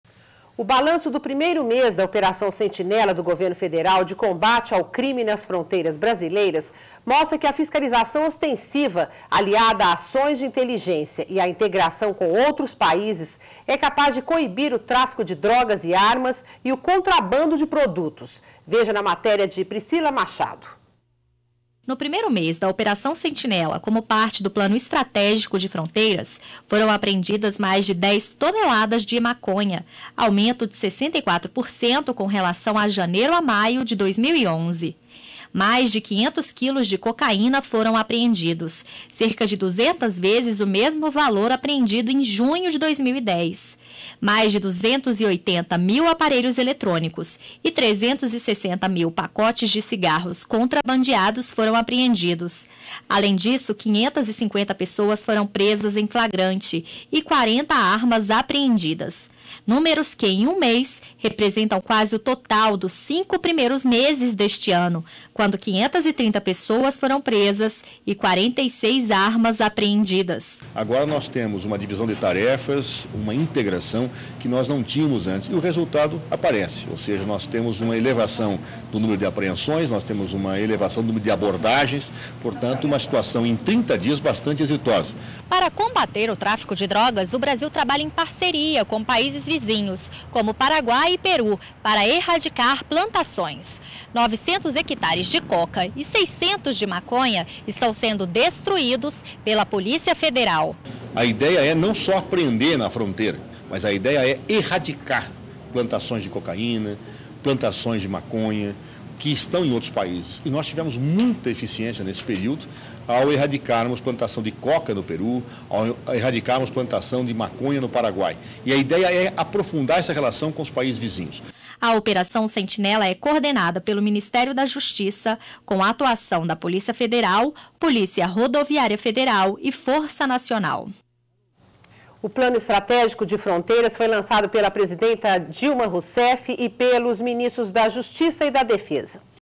Operação Sentinela reduz tráfico de drogas, armas e contrabando de produtos
NBR NOTÍCIAS - 11.07.11: O balanço do primeiro mês da Operação Sentinela do governo federal, de combate ao crime nas fronteiras brasileiras mostra que a fiscalização ostensiva, aliada a ações de inteligência e a integração com outros países, é capaz de coibir  o tráfico de drogas e armas e o contrabando de produtos.